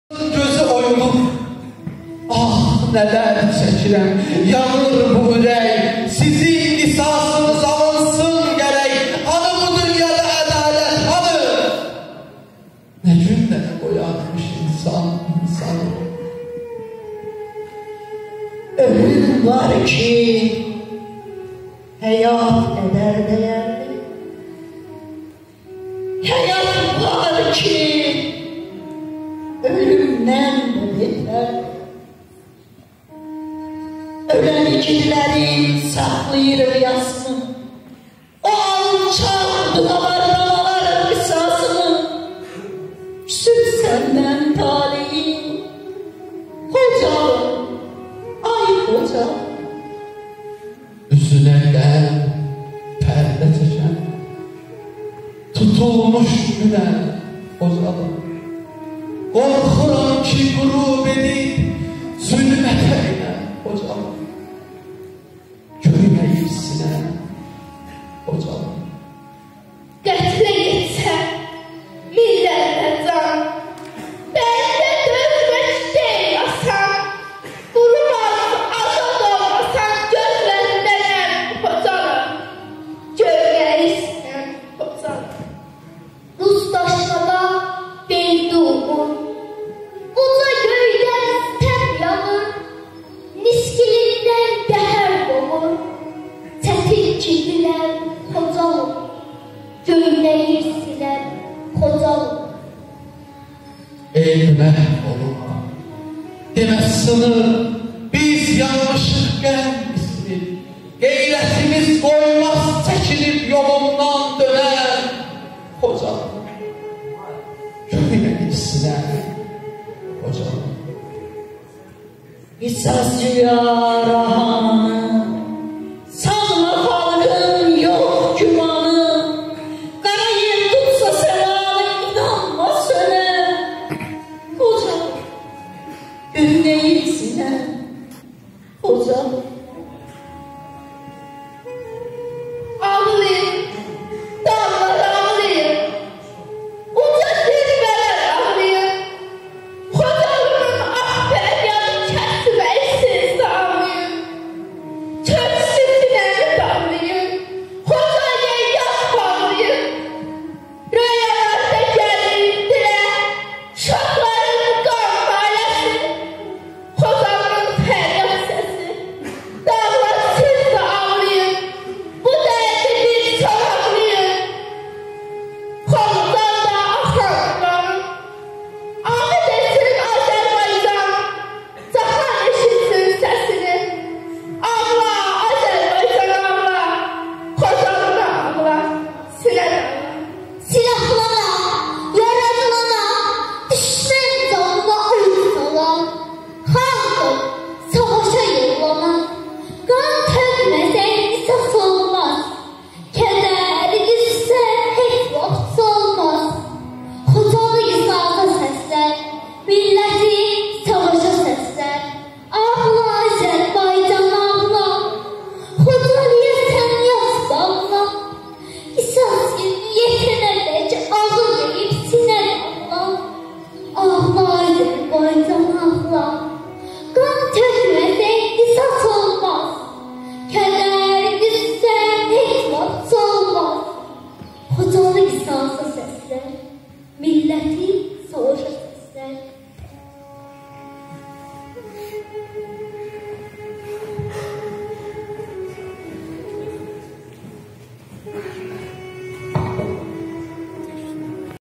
Semkir Kur medeniyyet Xocalı faciəsinin iyirmisəkkizinci ilinə həsr olunmuş anım tədbiri.
Orxan, Rəfiqə, Turan, Şəhla, Hicran.